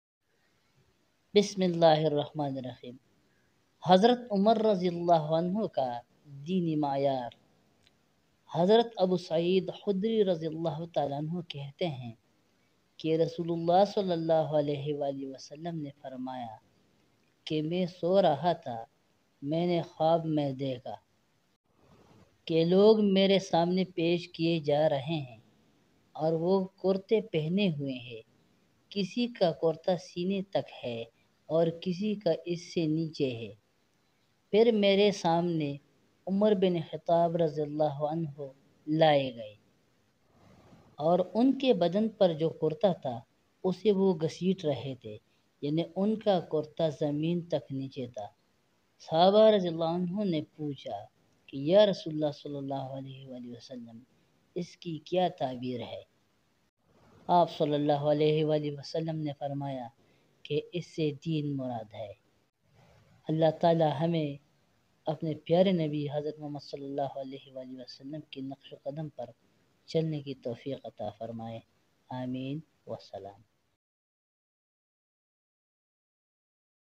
Umar RA ka Dini mayar || Hadith Mubarak || Sahih Bukhari || Gul Lala Studio
This is 23rd Hadith in the book Bokhari Sharif written by Imam Bokhari (Rehmat Ullah Alaih). This Hadith is about the...